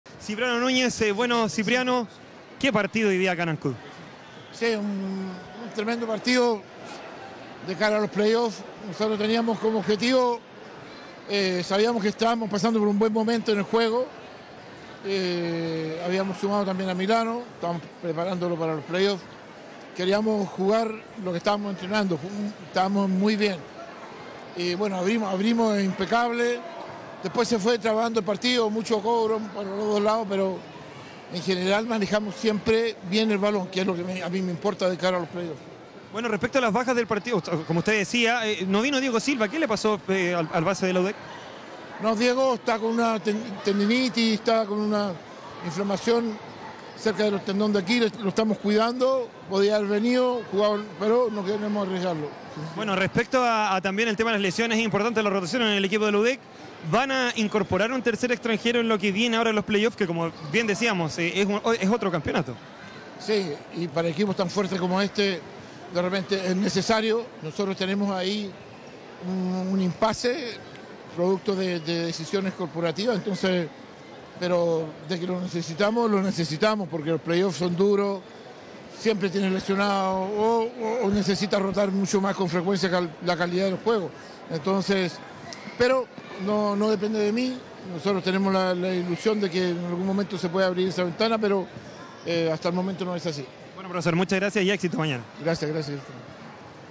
Postpartido: Cipriano Núñez, DT UdeC - 03 de mayo 2024
Cipriano Núñez, técnico de UdeC, conversa con CDO tras derrotar a ABA Ancud por la Liga JugaBet by Cecinas Llanquihue.